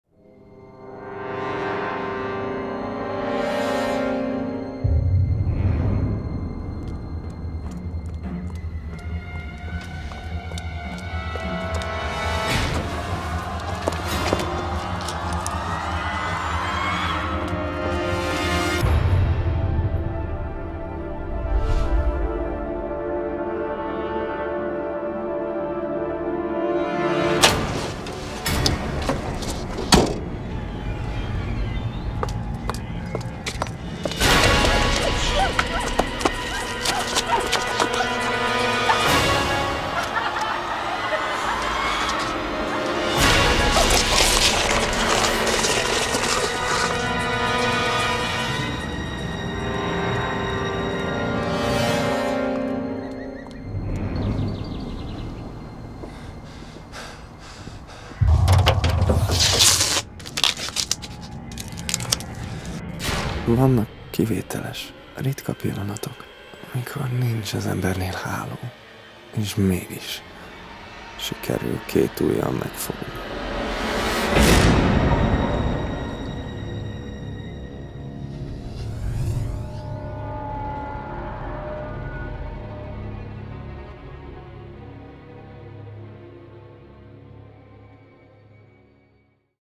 A lepkegyűjtő (színdarab) - trailer
A lepkegyűjtő című előadás 2015. április 11-től látható a Centrál Színházban.

Szereplők: Bereczki Zoltán és Ágoston Katalin

Rendező, zeneszerző: Horgas Ádám